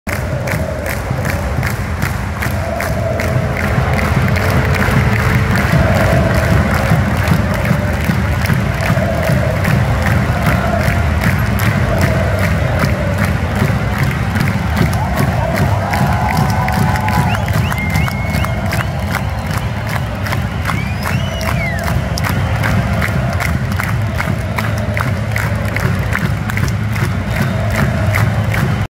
Abschied von Christian Streich- eine Legende sagt Adieu.
Christian Streich verabschiedet sich vor der Süd.